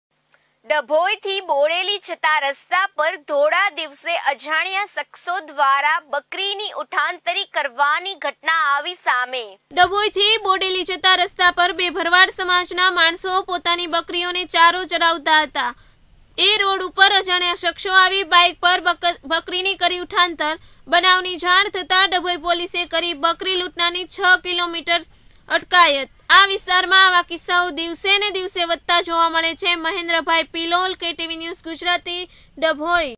ડભોઈથી બોડેલી જતા રસ્તા પર ધોળા દિવસે અજાણ્યા શખ્શો દ્વારા બકરીની ઉઠાંતર કરવાની ઘટનાઆવી સામે
ડભોઈથી બોડેલી
જતા રસ્તા પર ધોળા
દિવસે અજાણ્યા શખ્શો
દ્વારા બકરીની ઉઠાંતર
કરવાની ઘટનાઆવી
સામે